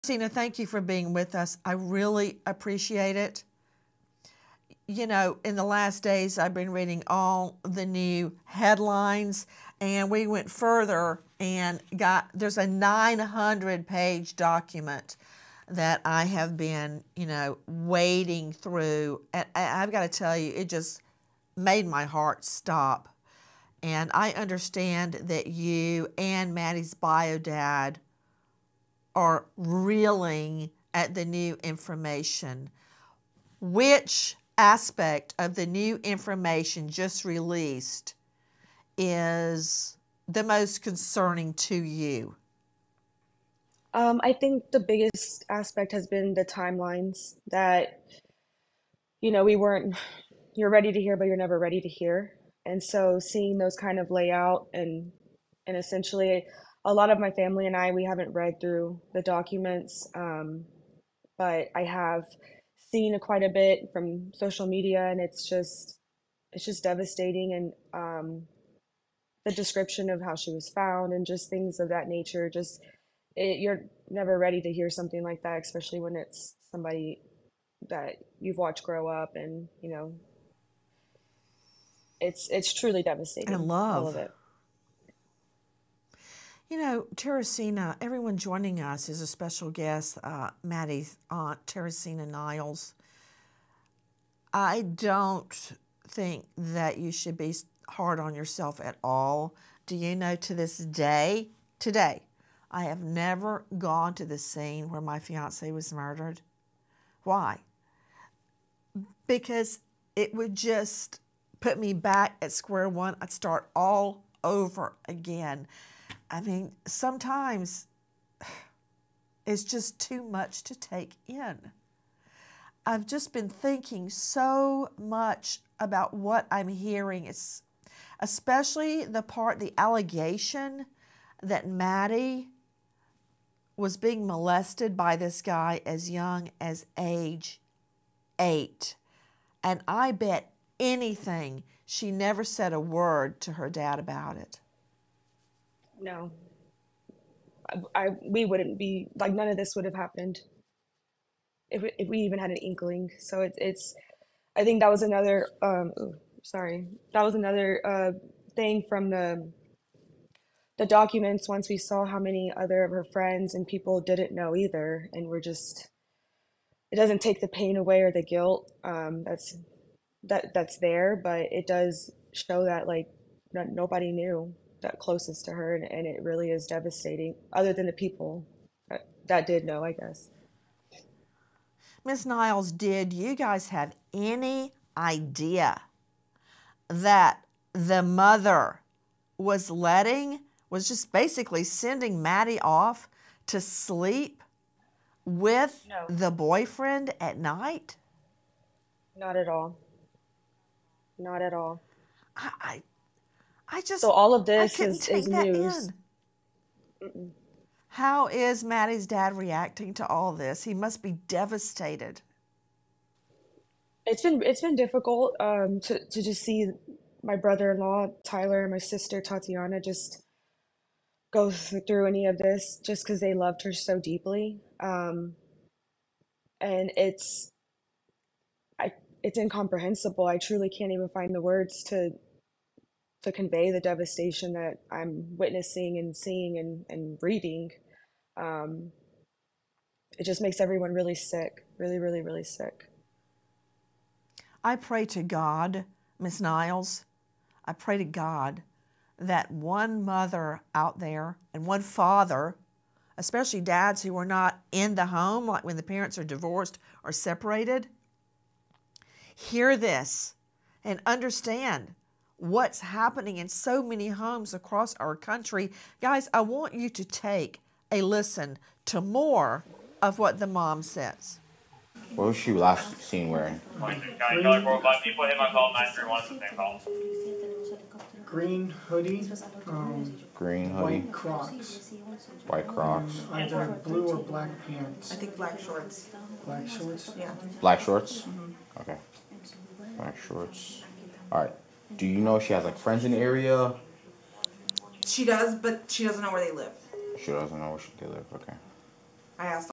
Madeline Soto's Aunt: She Never Spoke of Mom's Boyfriend's Abuse: "None of this Would Have Happened"
Florida investigators have released hundreds of pages relating to their investigation into the disappearance and murder of 13-year-old Madeline Soto, who was reported missing on February 26, the day after her birthday.

Soto’s body was found in a wooded area in Osceola County, near where her mother’s boyfriend was seen with a flat tire on the day of her disappearance, as CrimeOnline reported. The boyfriend, Stephan Sterns, said he dropped her off at school that morning, but investigators later determined that never happened and charged him with her murder.